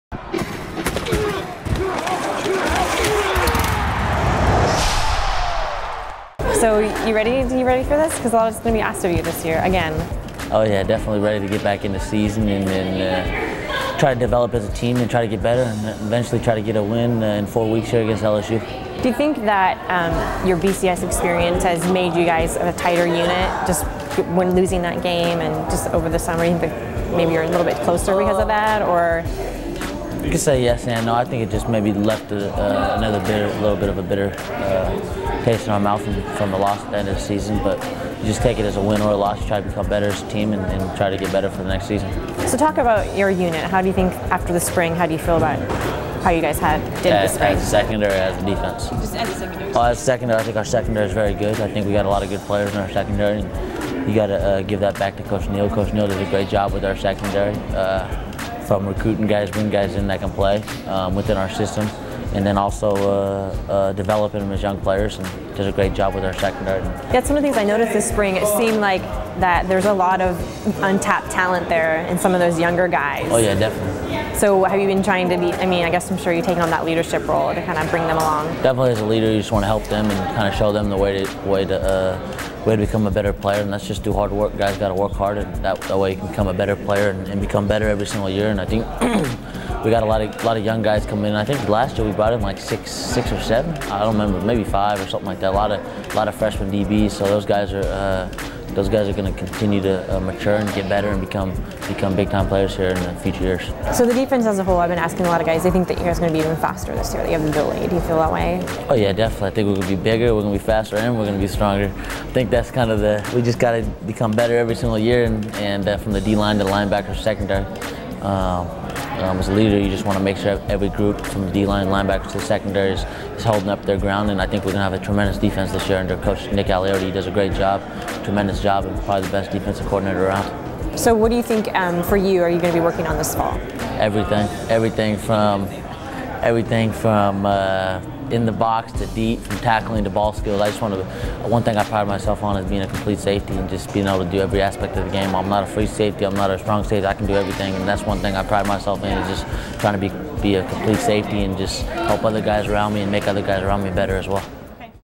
John Boyett Interview
John Boyett talks about the upcoming season at Oregon Football Media Day 2011